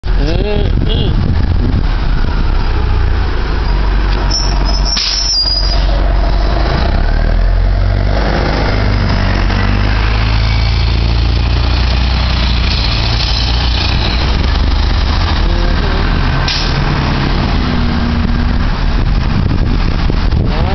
CTS #827
This bus was on the 9 line (Charlottesville High School-Downtown) line. Recorded at the 14th St./Grady Ave. intersection.